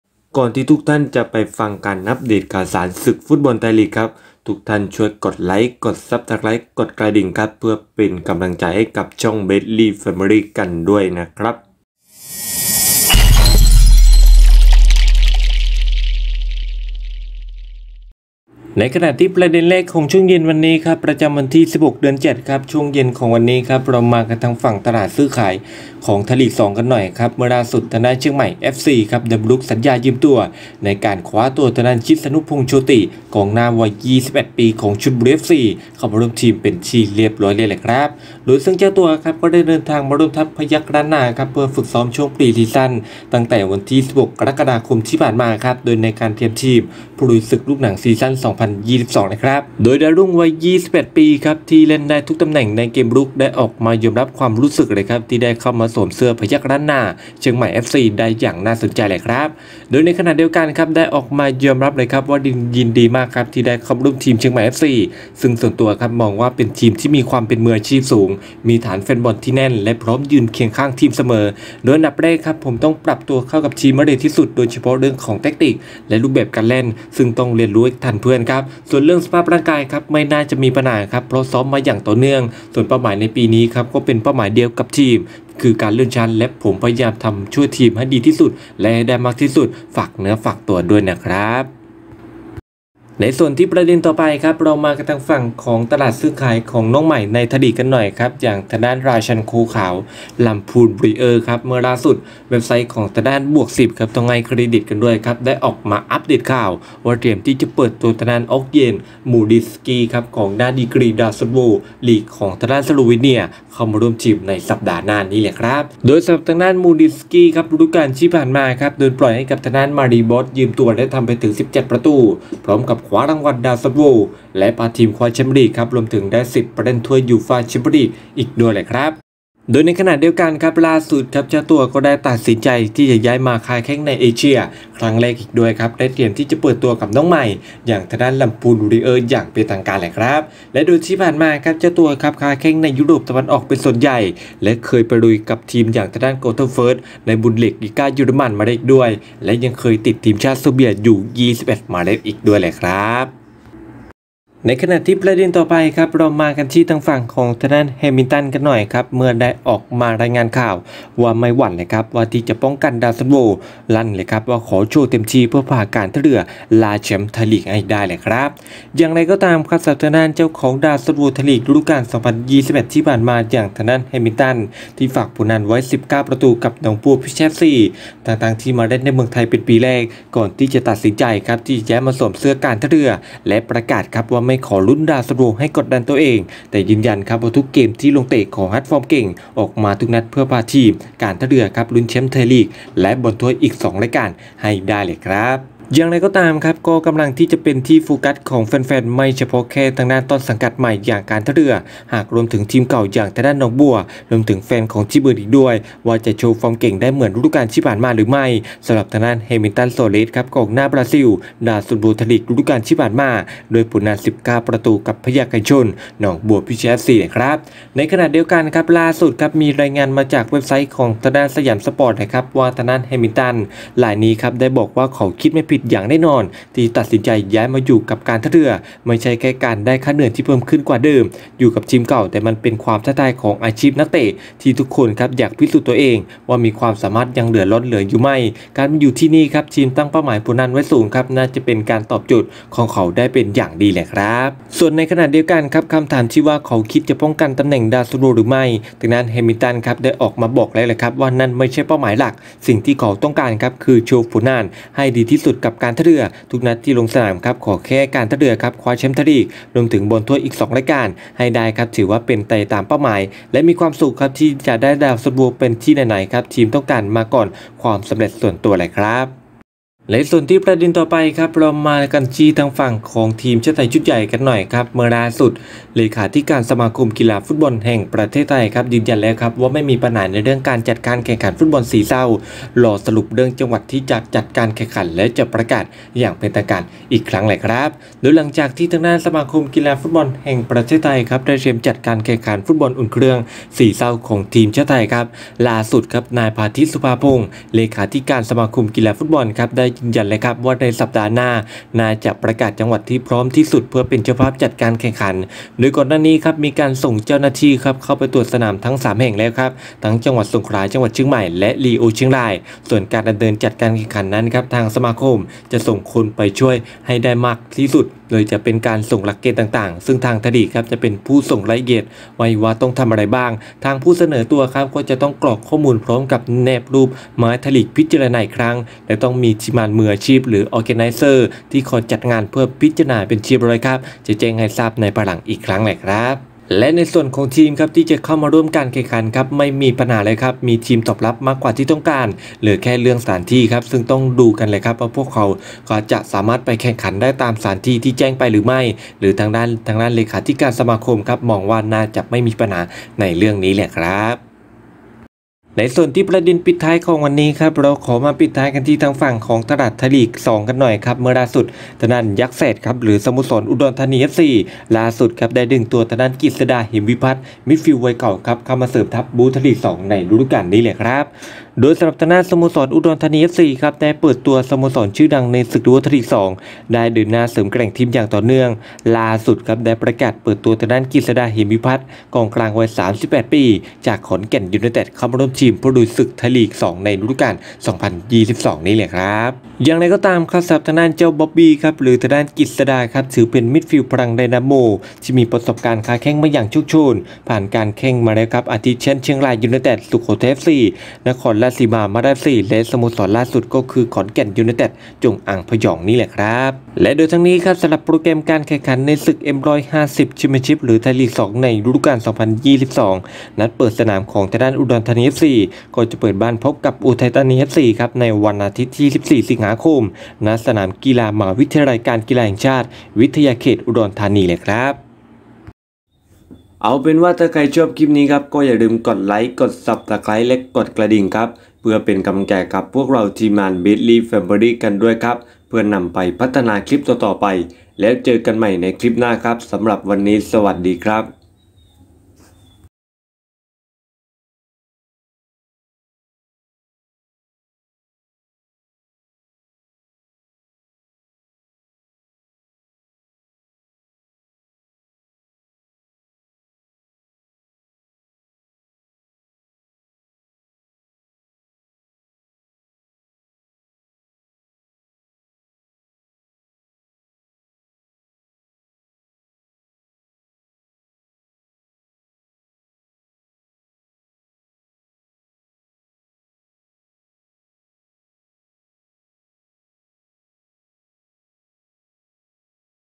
ลำพูน เตรียมคว้าดาวซัลโวลีกสโลวิเนีย, ชิษณุพงษ์ ย้ายซบเชียงใหม่ FC, แฮมิลตัน ลั่นคิดไม่ผิดซบท่าเรือ
ประเด็นข่าวในวันนี้
1. ลำพูน เตรียมคว้าดาวซัลโวลีกสโลวิเนีย
2. ชิษณุพงษ์ ย้ายซบเชียงใหม่ FC
3.  แฮมิลตัน ลั่นคิดไม่ผิดซบท่าเรือ
4. พาทิศ รับทีมชาติที่จะอุ่นเครื่องกับช้างศึกตอบรับแล้ว
5. อุดรธานี เปืดตัวกฤษดา เหมวิพัฒน์ ร่วมทัพ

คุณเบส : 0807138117
ติดตามเราได้อีกช่องทาง เพจ facebook : เบสลี่ Family V.2

เครดิตของข่าว